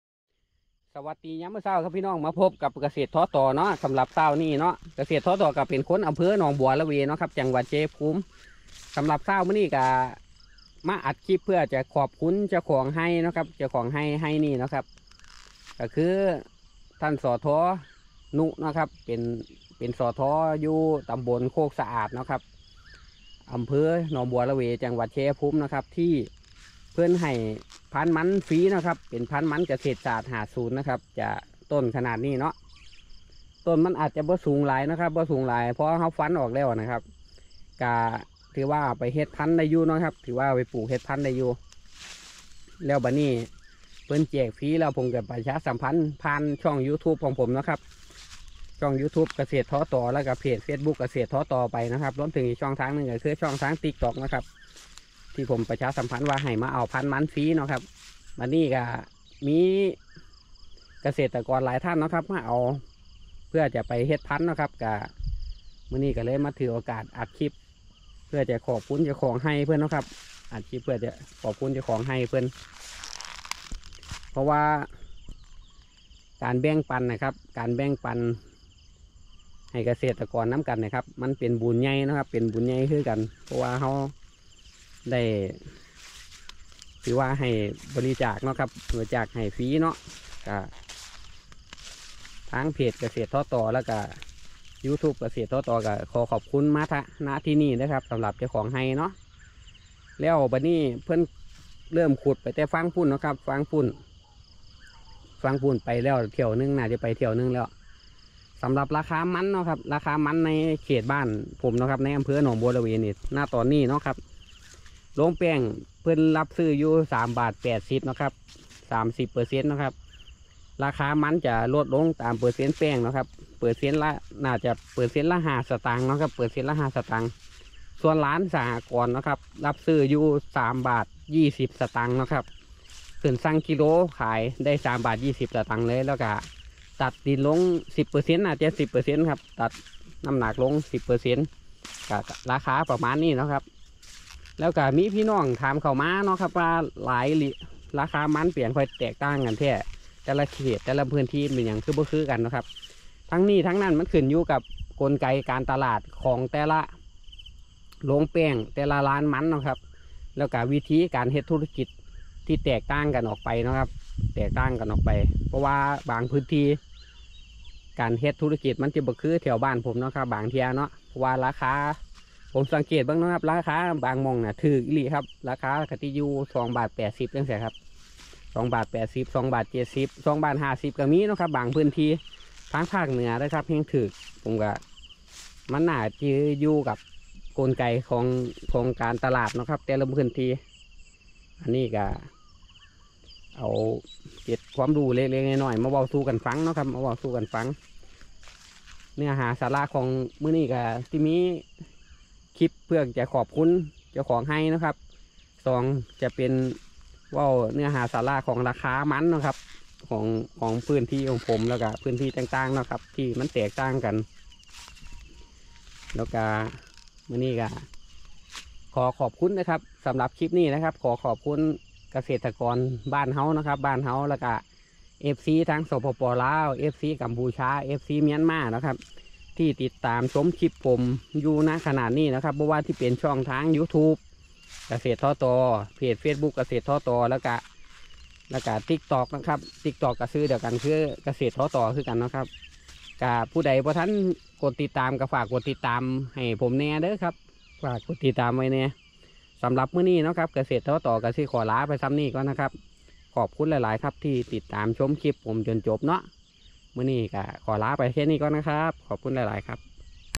ขอบคุณ สท.ณุ ที่แบ่งพันธุ์เกษตรศาสตร์50 ให้กับชาวไร่มันครับ โทรศัพท์
ขอบคุณ สท.ณุ ที่แบ่งพันธุ์เกษตรศาสตร์50 ให้กับชาวไร่มันครับ